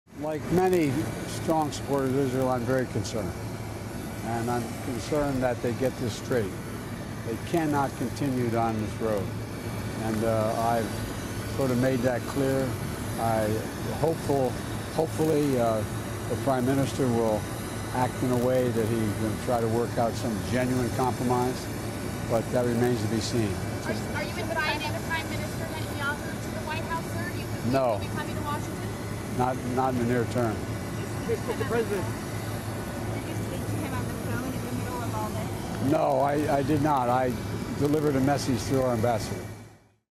Joe Biden scorns Netanyahu over judicial reform plans
The US President said he was very concerned about political turmoil in Israel and would not invite the country's prime minister to the White House in the near term.